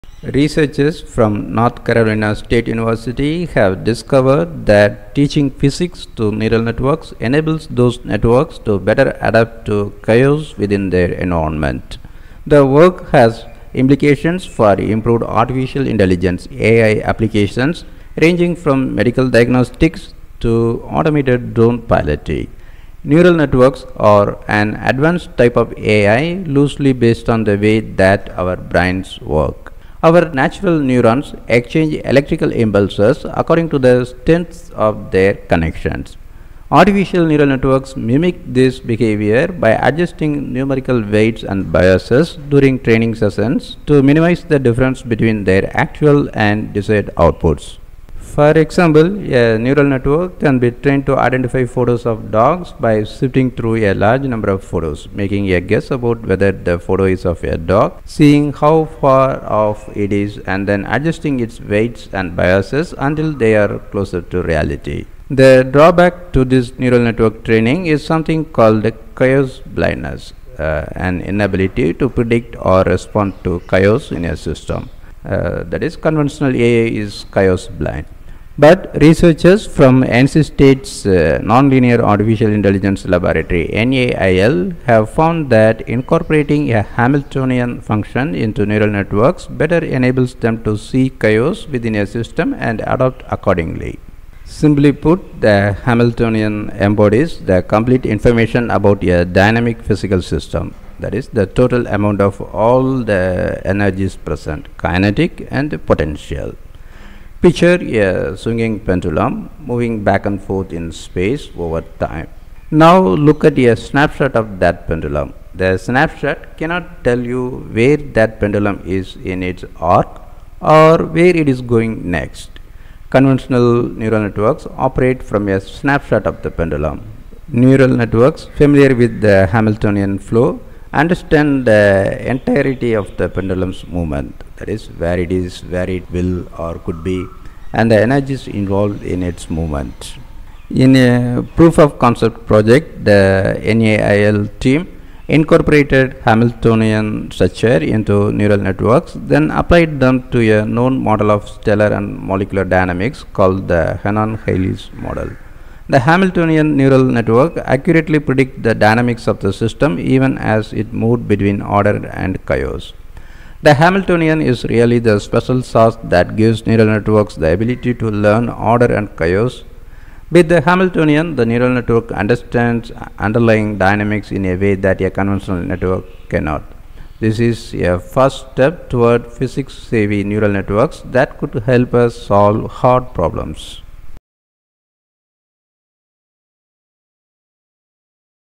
Teaching Physics to Neural Networks Removes ‘Chaos Blindness’
Researchers from North Carolina State University have discovered that teaching physics to neural networks enables those networks to better adapt to chaos within their environment. The work has implications for improved artificial intelligence (AI) applications ranging from medical diagnostics to automated drone piloting.